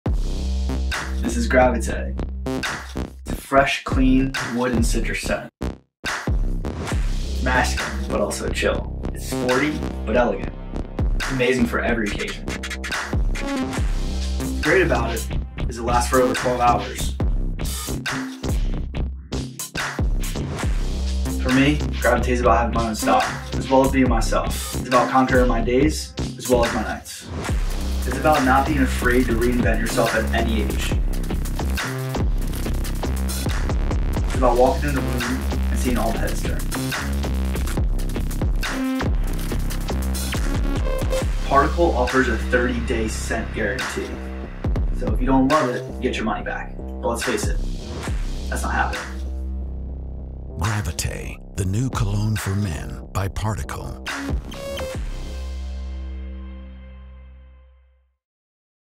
Your Scene, Our Scent: Particle For Men Gravité | Particle For Men Skincare & Haircare
Set the scene for passion as you take charge of the destination, and let Particle's Gravité cologne be your sensual accomplice. Crafted for the discerning man, Gravité intertwines the richness of wood, the zest of citrus, the allure of papyrus, and more. This handcrafted elixir embodies a fresh and clean masculinity, an irresistible scent destined to elevate every event to a realm of seductive sophistication.